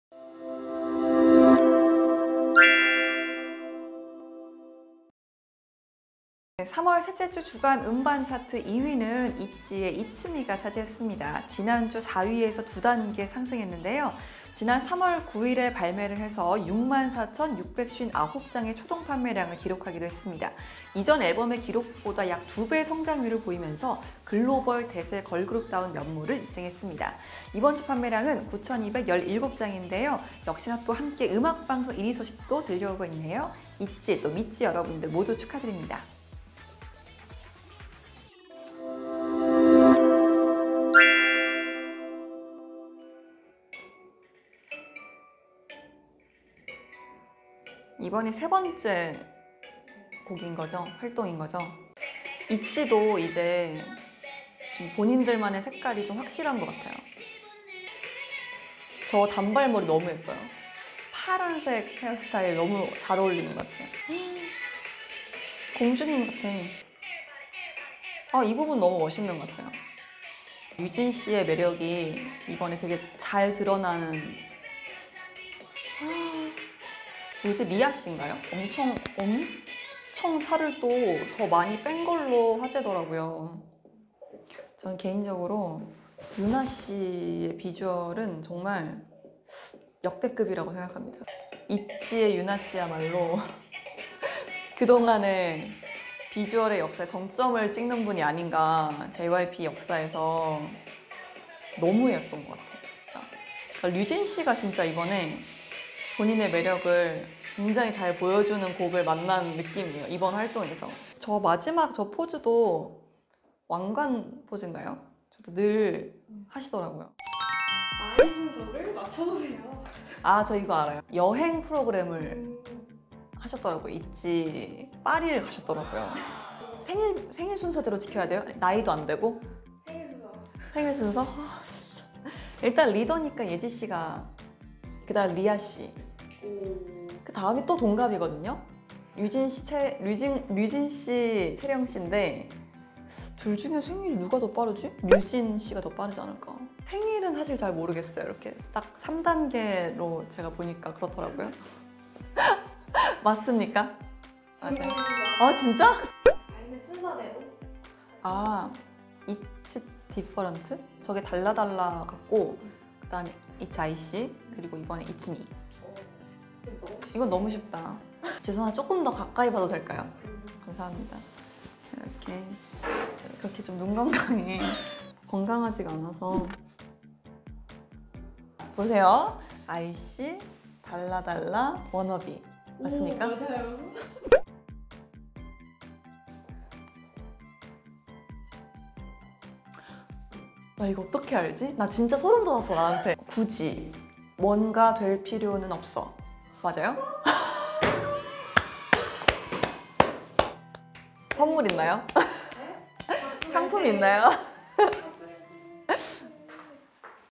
ITZY QUIZ and Album Sales 3rd week of March
Weekly K-POP Album Chart TOP 10 - Mar 2020 week 3

This album sales based on the world's only the real time music chart,
HANTEO Physical Album Chart.